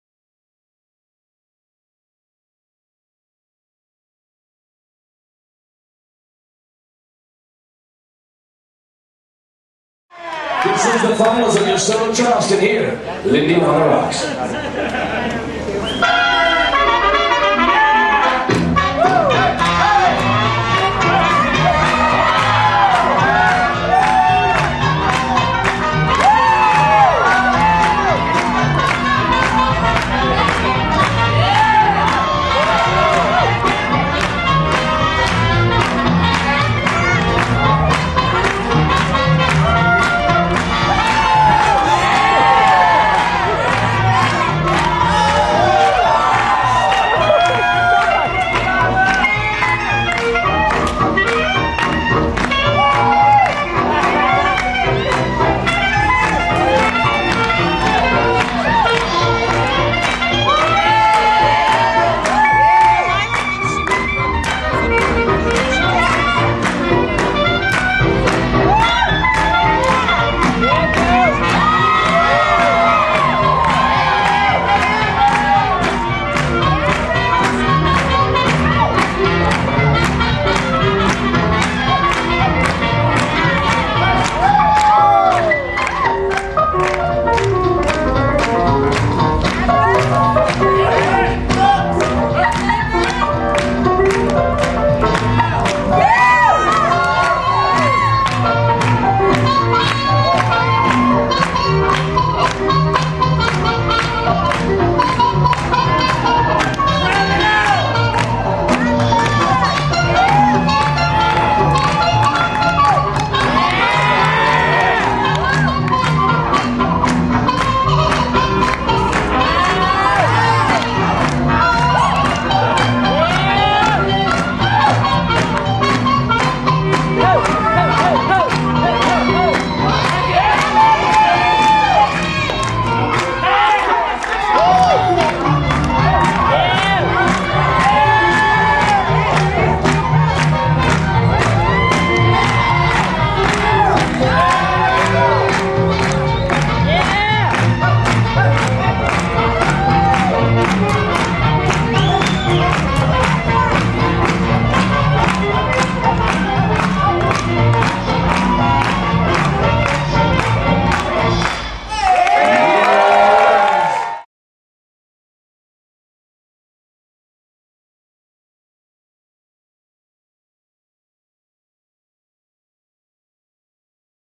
Lindy on the Rocks (LOTR) 2010 - Solo Charleston Finals (HD)
to download the High Quality and High Definition video.

Lindy on the Rocks (LOTR) 2010 - held in Denver, Colorado - Solo Charleston Finals Competition on Saturday, August 7, 2010 (Angle: HC7A, Left Angle).

Contest Winner: Joanna Lucero